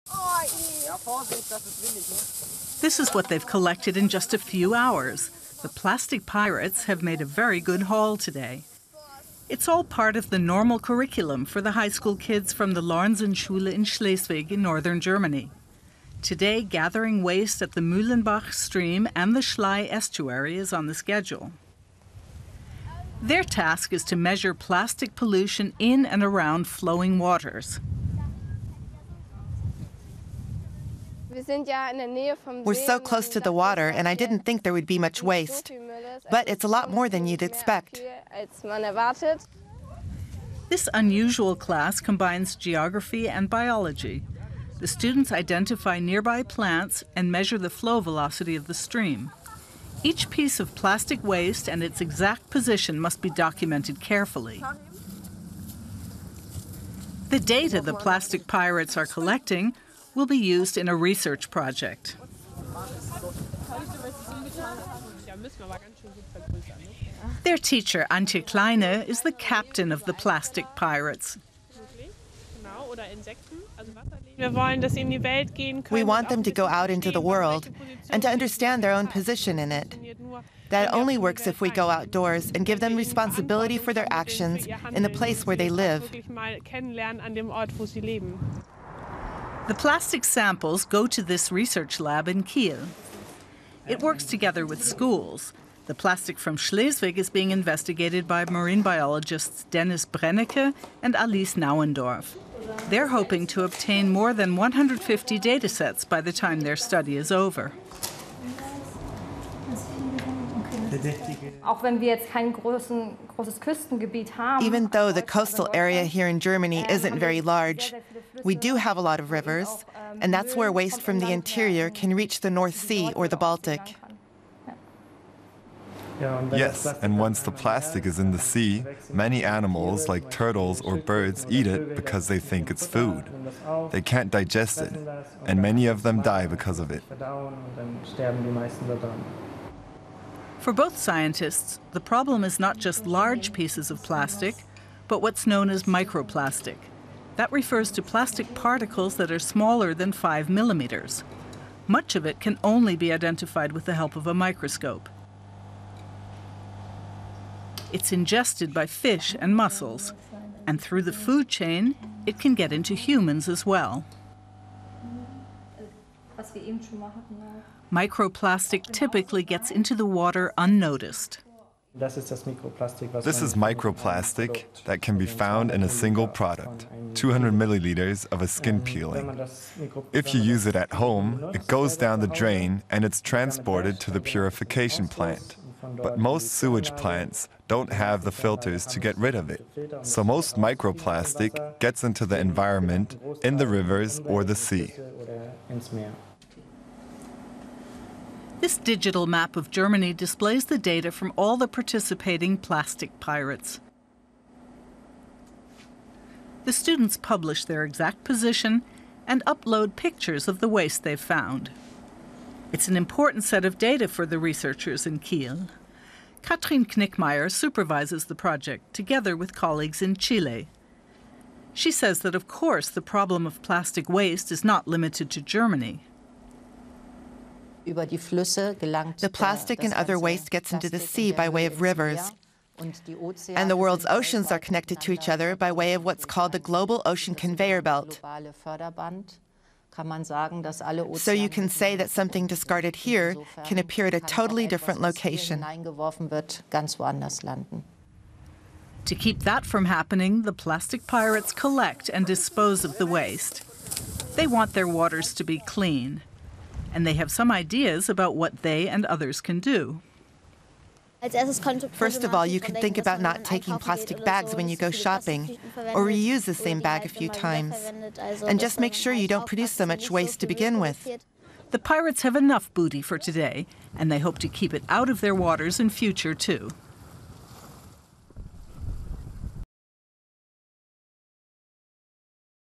The plastic pirates | Eco-at-Africa
These environmental buccaneers aren't after gold – they’re school kids who hunt for plastic waste, cleaning rivers and beaches in northern Germany, and collecting scientific data as they go.